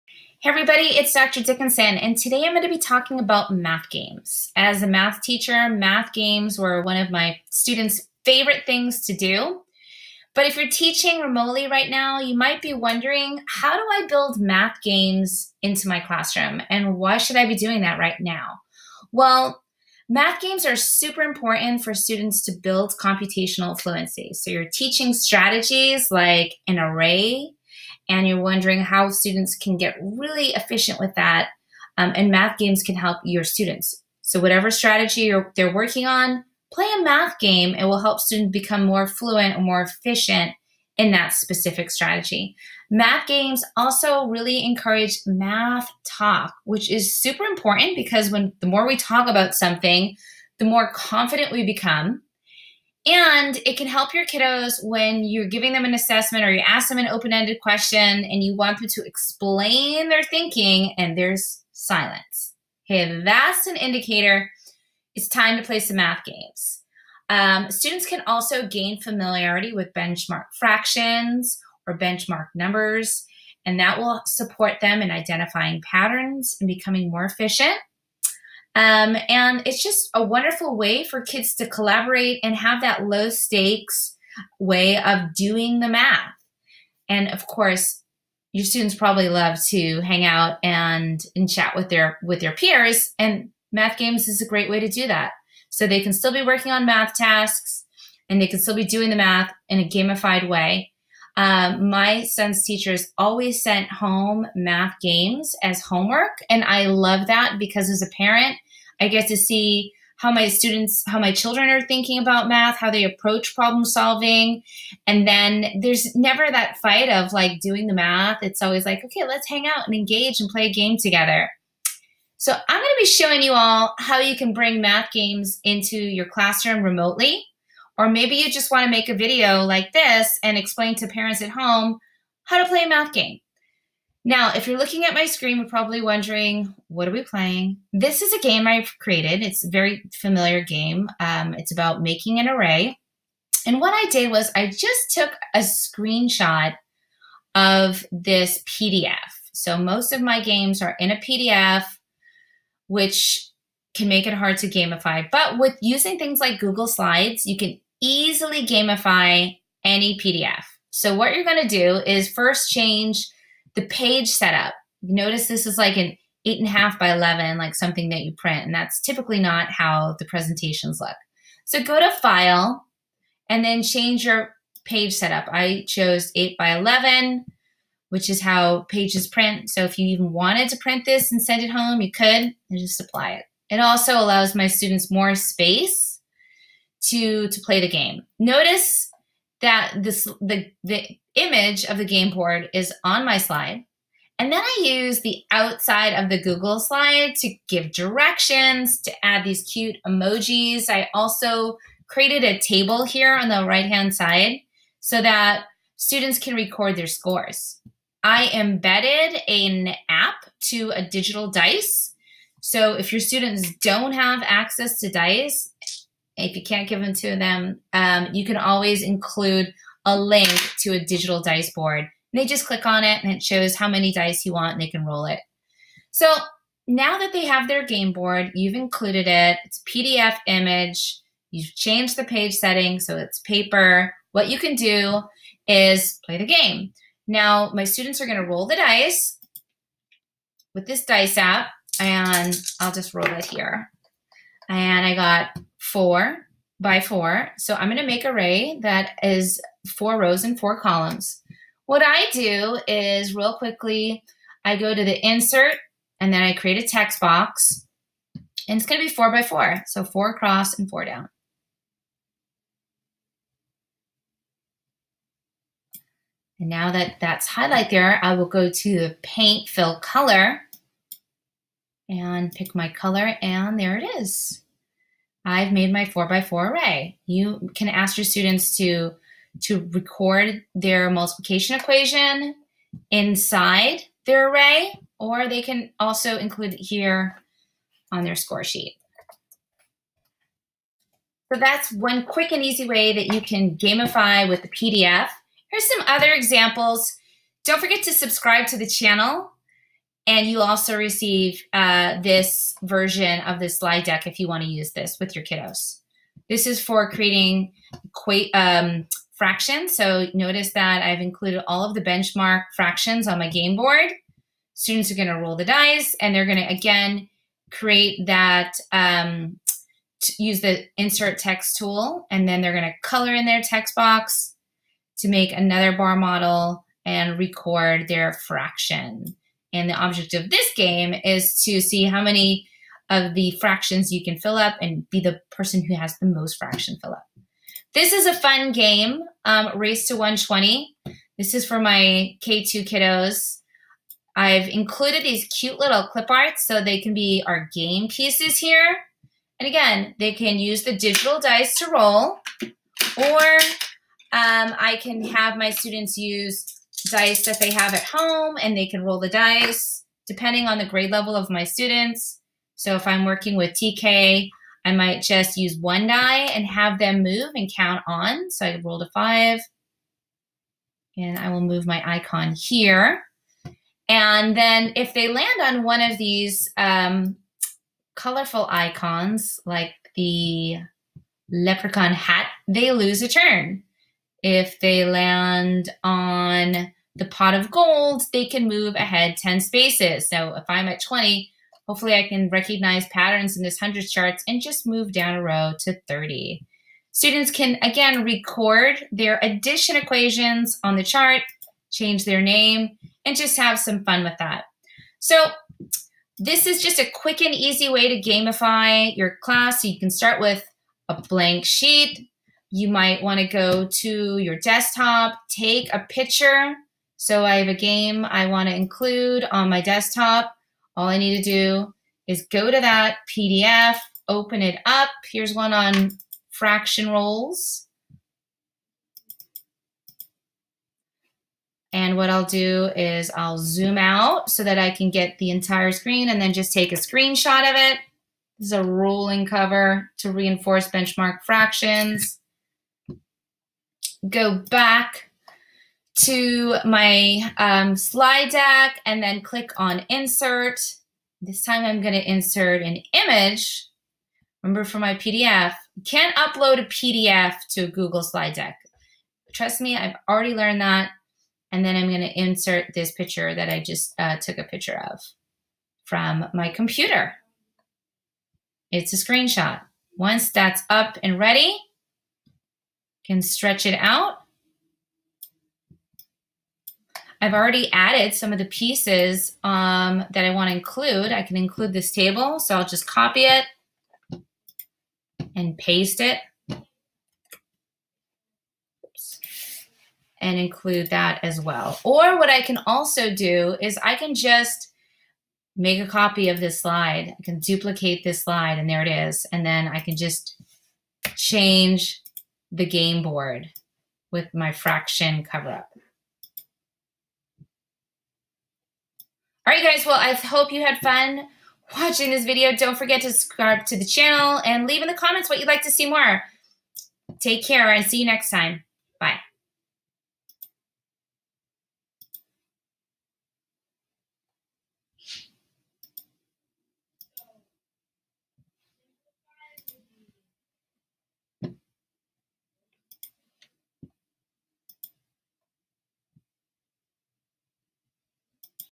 How to Use Google Slides To Create Math Games for K-12 Using Quick & Easy PDF Docs.
In this video, Dr. Dickenson shares how to create games on Google Slides from PDF.  She shares tips and strategies to make a game and include digital tools and a place for students to record their score.  Learn how to make your online instruction engaging with a gamified approach so students are working collaboratively during remote instruction. You can't miss this video!